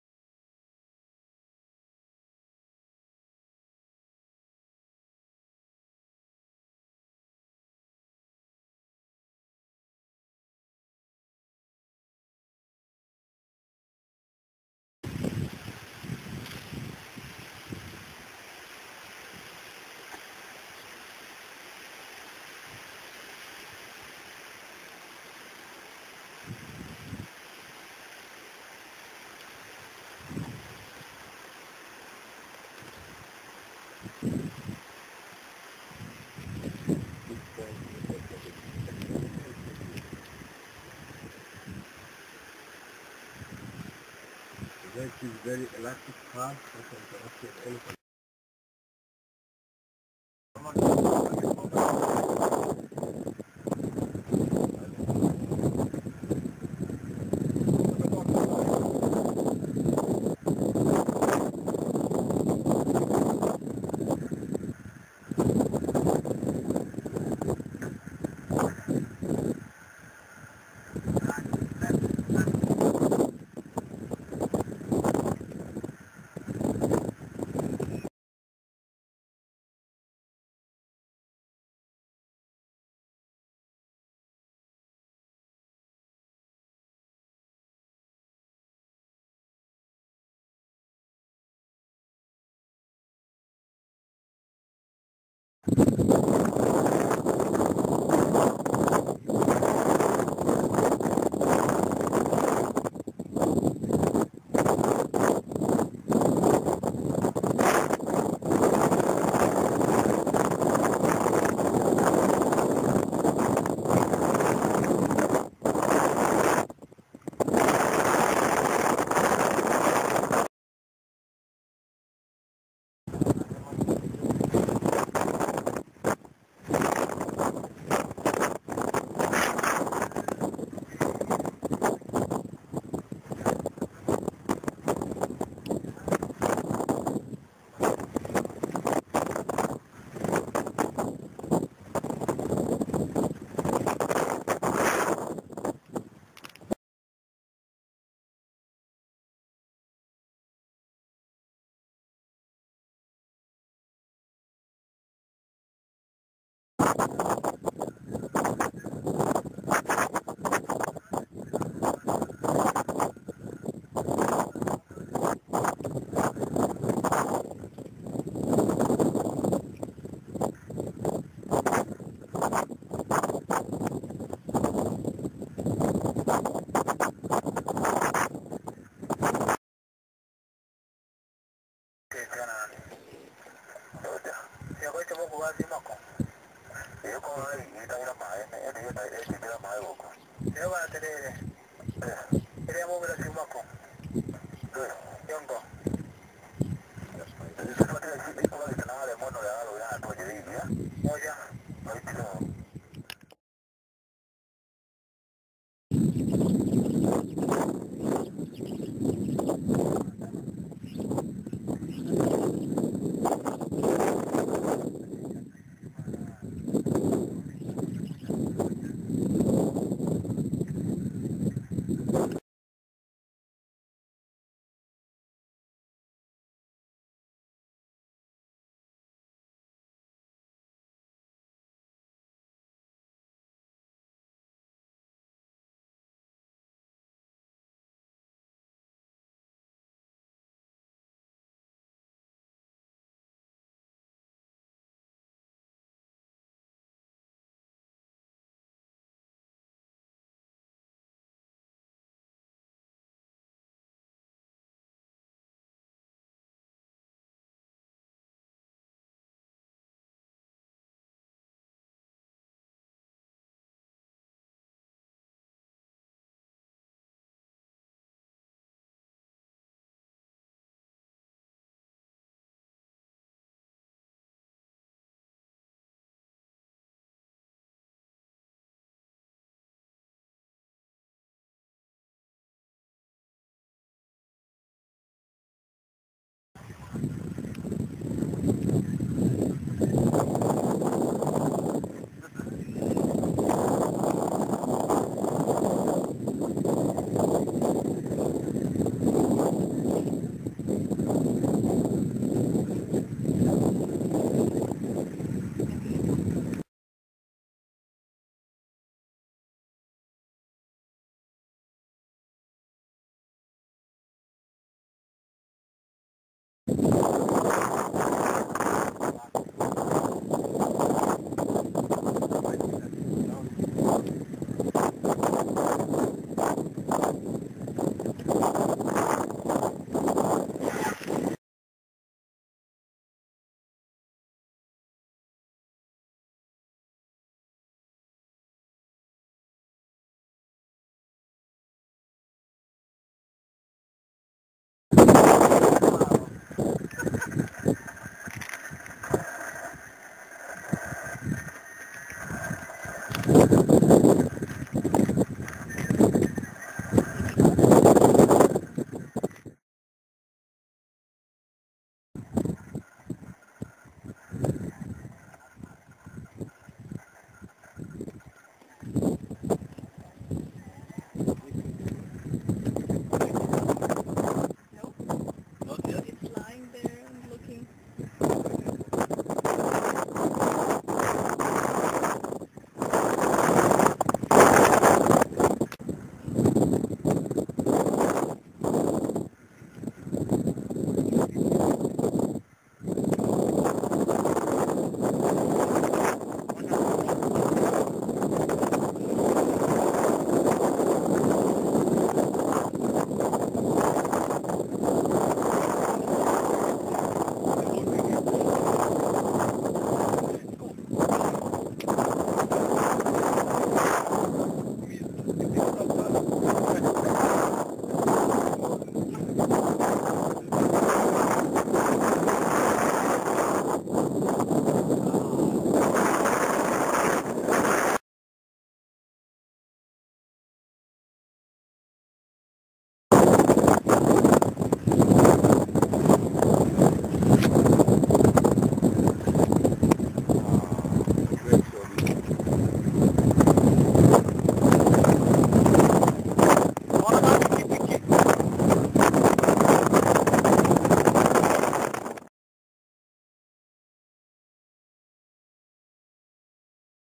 Samburu National Park / Kenya - game drive 2
great scenes at the end of the clip, just before night came in, we were lucky to see.